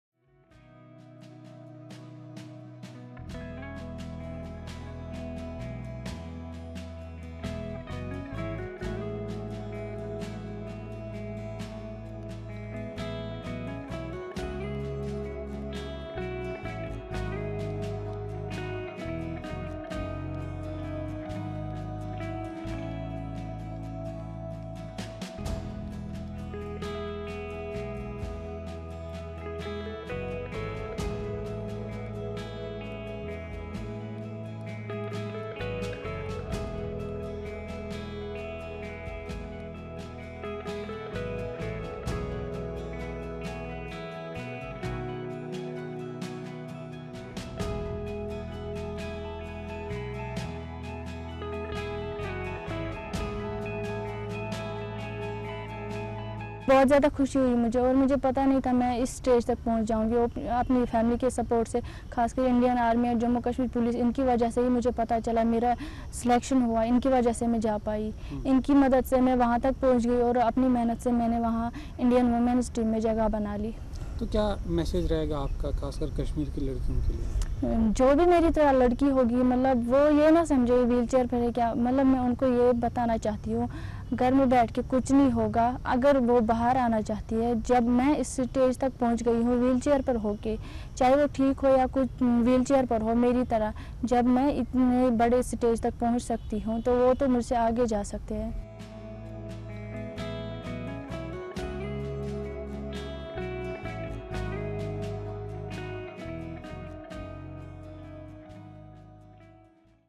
Ishrat Rashid is the first Kashmiri specially-abled Basketball player to play at International level
Ishrat Rashid is the first Kashmiri specially-abled Basketball player to play in the International Wheelchair Basketball Tournament. Ishrat Rashid said that she would want every girl to come out of their homes chaise their dreams. Ishrat injured her spinal cord in an accident in 2016. After the accident, she was bedridden for eight months. She eventually joined the Voluntary Medicare Society in Srinagar, where she was introduced to wheelchair basketball. The Thailand Wheelchair Basketball Association is hosting the IWBF 2019 Asia Oceania Championship. Fifteen countries from the Asia Oceania region will be represented at the IWBF 2019 Asia Oceania Championships that will be held in Thailand's Pattaya.  The Wheelchair Basketball tournament will be a grand event featuring fourteen men's teams, eight women's teams and will be held from 29 November till 7 December.